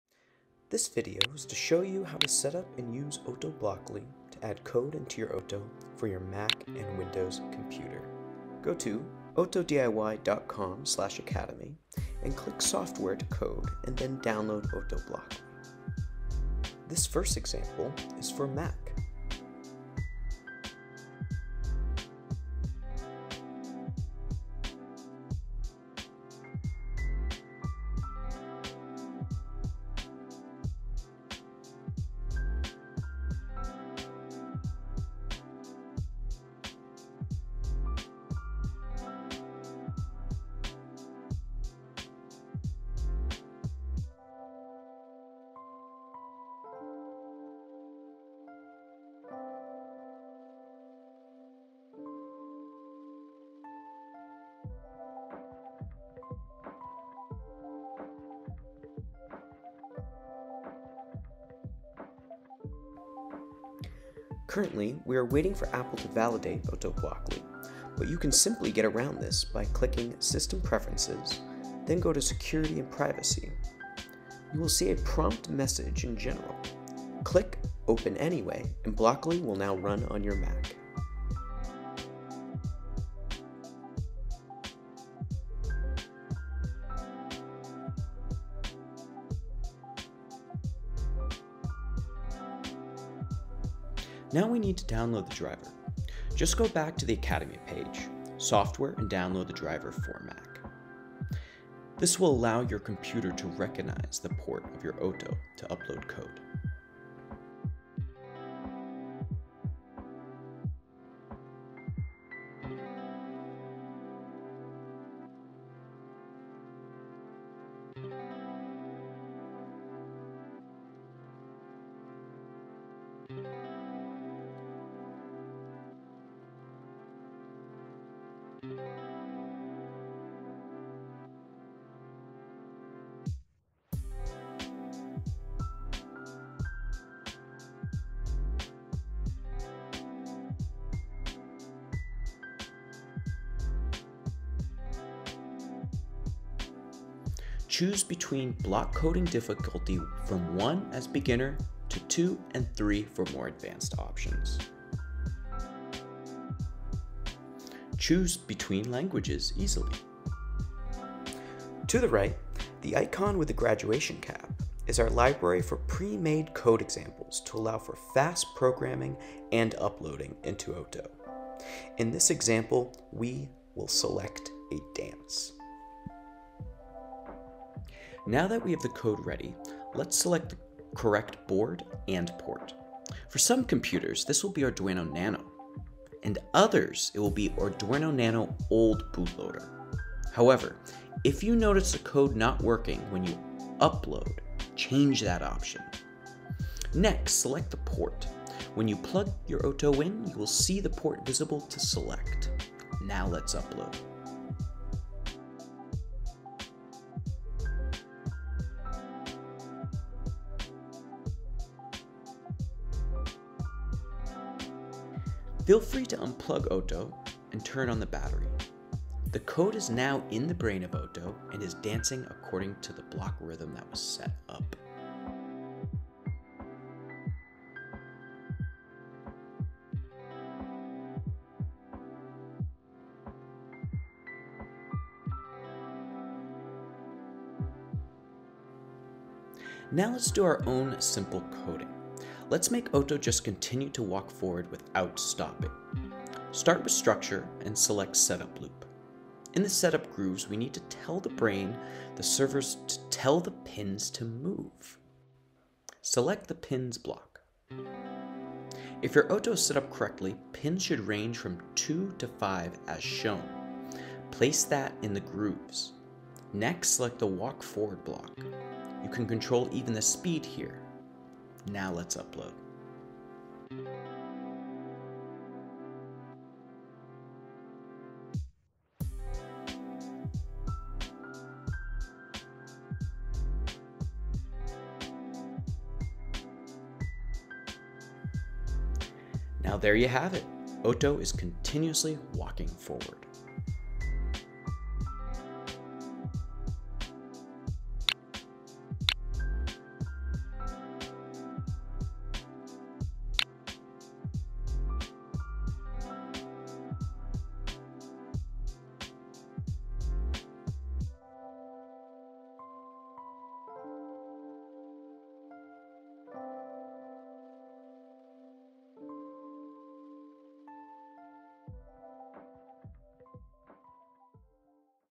How to install Otto Blockly and drivers in Mac and Windows computers and code your Arduino robots
Otto Blockly is a free and open source visual programming language based on Blockly from Google & MIT, to generate C/C++ code, compile and upload to any Arduino. Compatible with any OttoDIY robot or Arduino related boards. It is the perfect software to get you started into coding and STEM robotics.